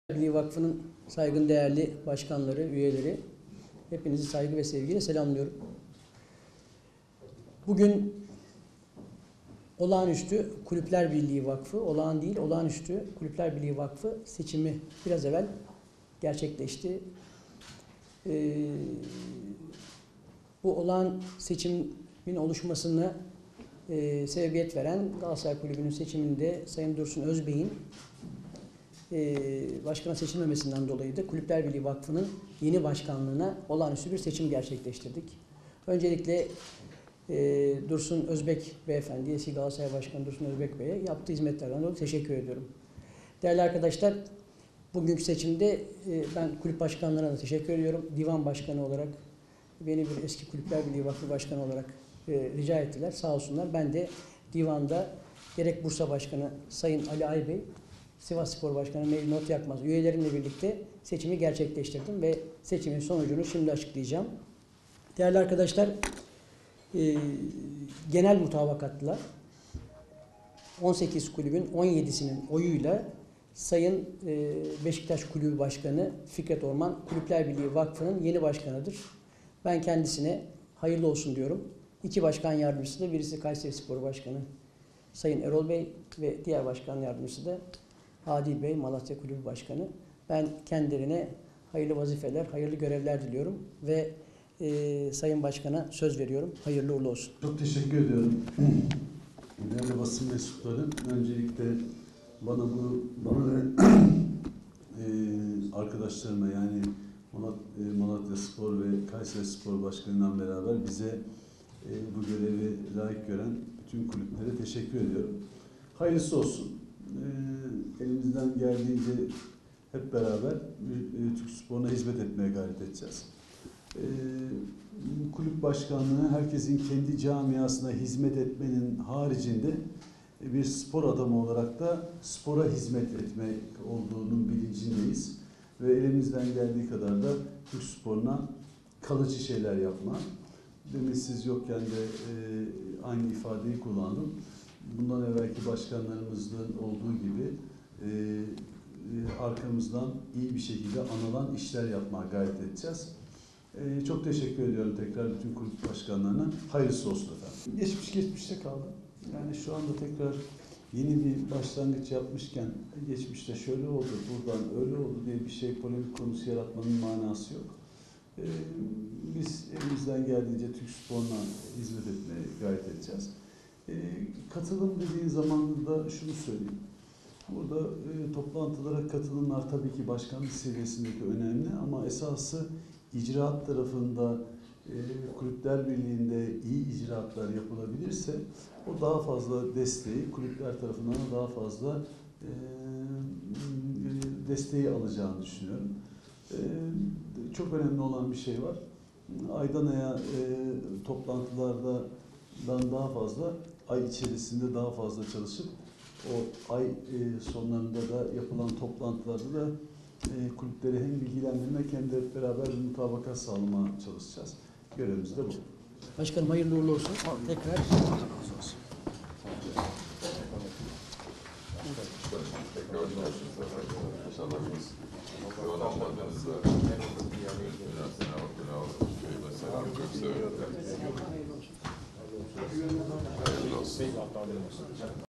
Kulüpler Birliği’nde Fikret Orman Dönemi
Kulüpler Birliği’nin yeni Başkanı Fikret Orman geçmişin geçmişte kaldığını ifade ederek, “Kulüpler yeni bir başlangıç yapmışken geçmişi kurcalamanın anlamı yok” diye konuştu.

İstanbul’da gerçekleşen Kulüpler Birliği olağanüstü toplantısı sonrası başkanlığa seçilen Beşiktaş Kulübü Başkanı Fikret Orman seçildi. 18 Süper Lig kulübün 17’si Orman’a oy verirken, sadece Fenerbahçe çekimser oy kullandı. Başkan Orman, daha sonra basın mensuplarına açıklamalarda bulundu. Evkur Yeni Malatyaspor Başkanı Adil Gevrek

ve Kayserispor Başkanı Erol Bedir ile beraber bu görevi veren kulüplere teşekkür eden Orman, “Elimizden geldiğince Türk sporuna hizmet edeceğiz. Herkesin camiasına hizmet etmenin haricinde spora hizmet etmenin bilincindeyiz. Arkamızdan iyi bir şekilde anılan işler yapmaya devam edeceğiz” dedi.

“GEÇMİŞ GEÇMİŞTE KALDI”

Geçmişin geçmişte kaldığını belirten Başkan Orman, “Kulüpler yeni bir başlangıç yapmışken geçmişi kurcalamanın anlamı yok. Biz elimizden geldiğince hizmet vereceğiz. Toplantılara katılımın başkanlık seviyesinde olması önemli ama Kulüpler Birliği’nde icraatlar daha önemli. Bu durum kulüpler tarafından daha fazla destek alır. Aydan aya toplantılardan daha fazla çalışıp, kulüpleri daha iyi bilgilendirip, mutabakat sağlamaya çalışacağız” şeklinde konuştu.lgilendirip, mutabakat sağlamaya çalışacağız” şeklinde konuştu.